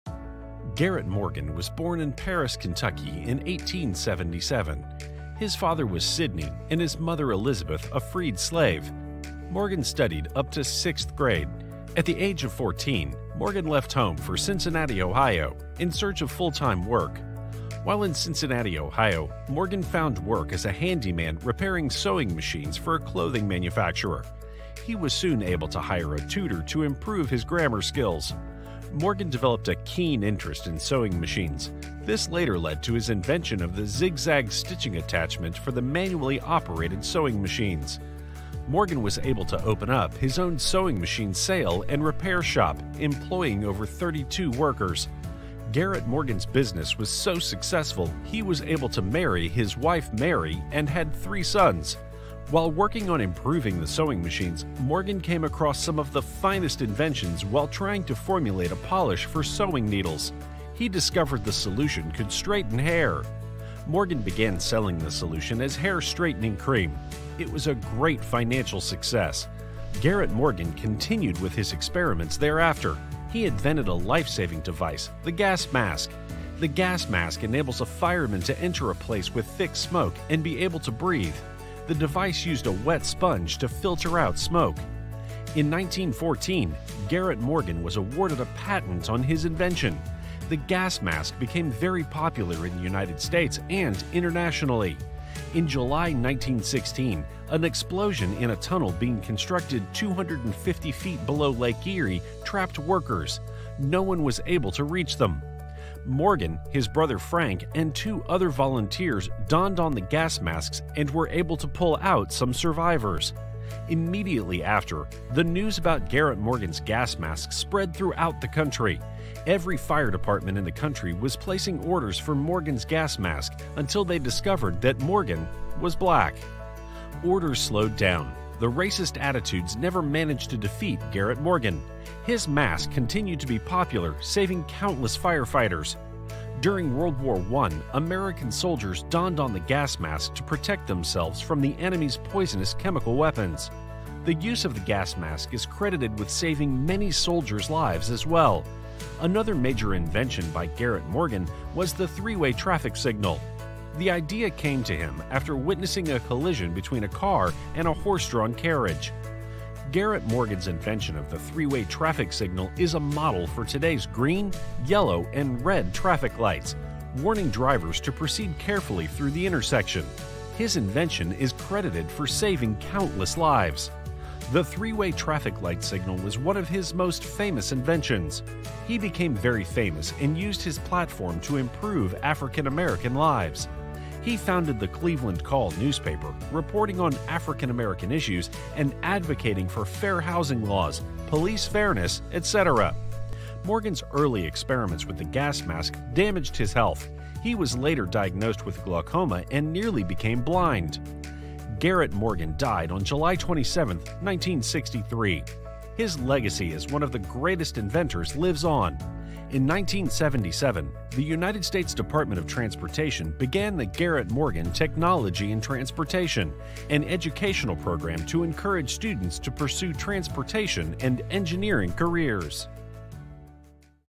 GARRET MORGAN
Garret Morgan was an African America who invented Life Saving devices, the gas mask used in World War 1, protecting soldiers from toxic gas. He received a patent for his invention in 1914. Garret also invented the first automatic three-way traffic signal system, he received a patent for his invention in 1923.